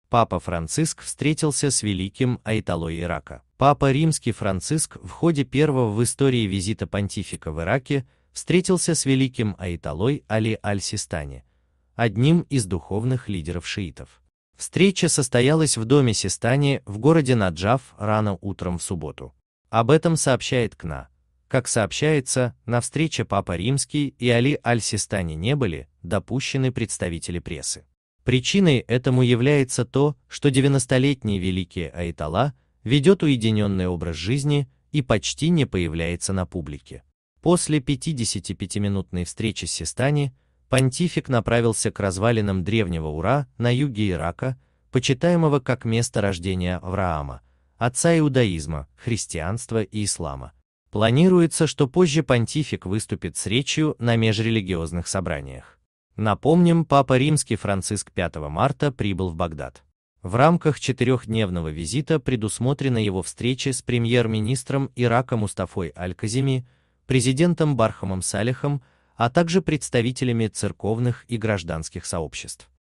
Папа Франциск встретился с великим аятоллой Ирака.
Новости Вмире ПапаРимский Папа Римский Франциск в ходе первого в истории визита понтифика в Ираке встретился с великим аятоллой Али аль-Систани, одним из духовных лидеров шиитов. Встреча состоялась в доме Систани в городе Наджаф рано утром в субботу. Об этом сообщает CNA. Как сообщается, на встрече Папа Римский и Али аль-Систани не были допущены представители прессы. Причиной этому является то, что 90-летний великий аятолла ведет уединенный образ жизни и почти не появляется на публике. После 55-минутной встречи с Систани п